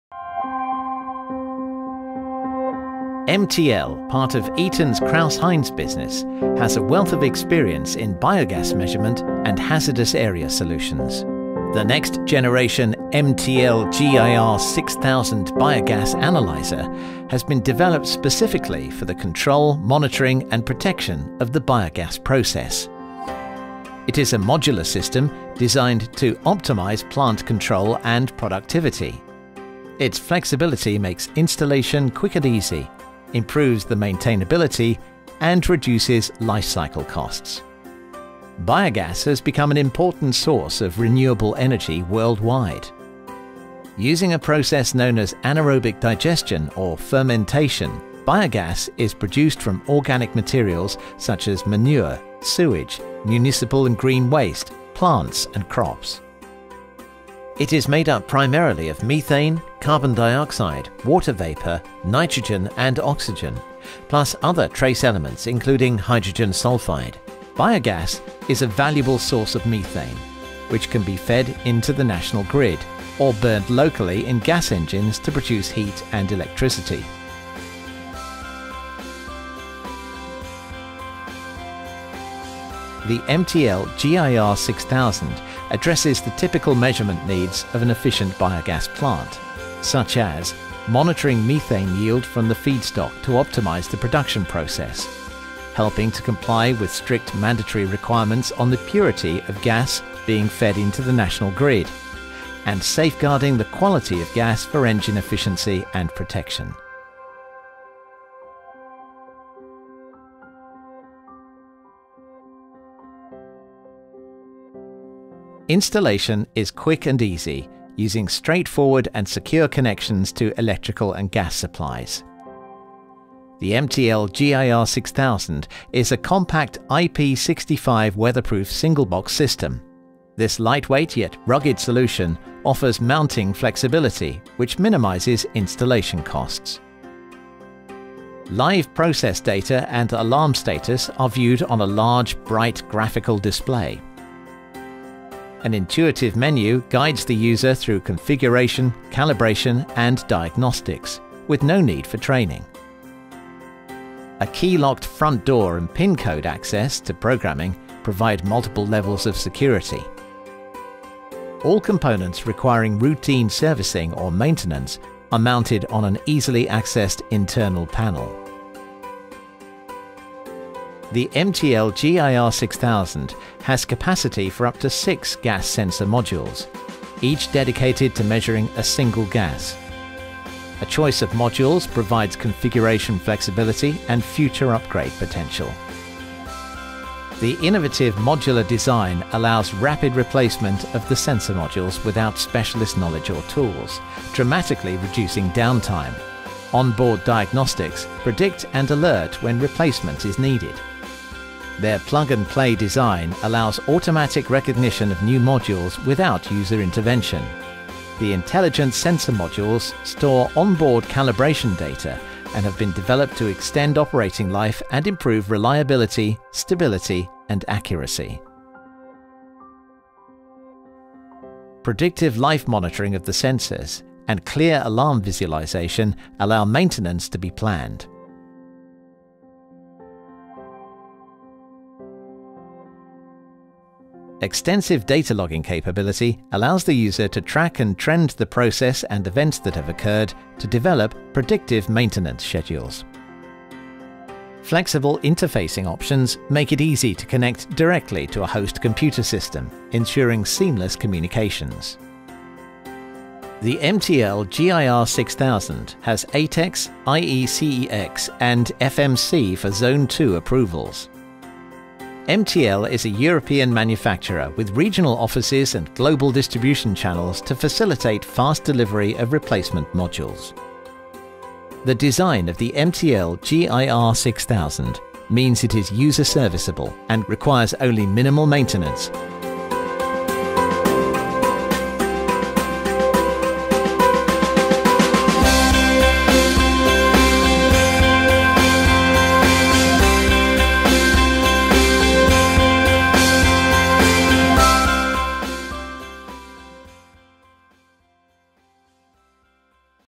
Plug in to the next generation of Biogas Analyser, MTL GIR6000.
This video introduces the new and innovative modular MTL GIR6000 Biogas analyser along with applications of a typical Biogas plant. Understand how this exciting new analyser can help you to optimise your plant efficiency in one quick and easy installation.